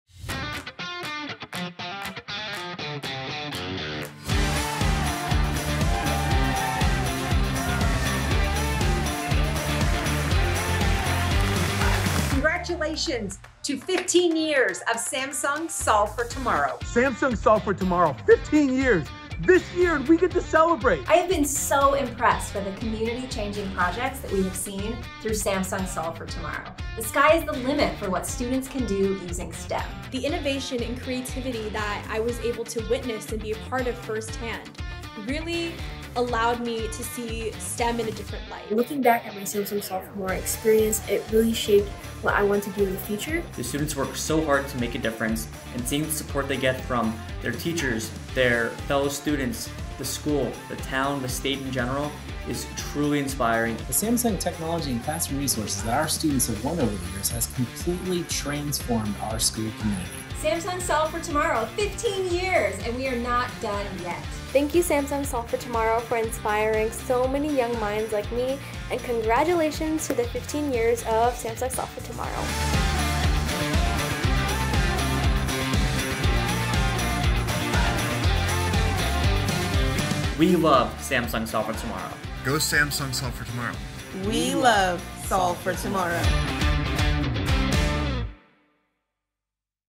Celebrating 15 years of Samsung Solve for Tomorrow!
Samsung Solve for Tomorrow is a nationwide STEM contest that gives public school students in grades 6-12 the chance to win a share of $2 million in technology and classroom supplies by creating a solution to a community problem. Now entering and celebrating its 15th year take a look back at some of our highlights and hear from alumni!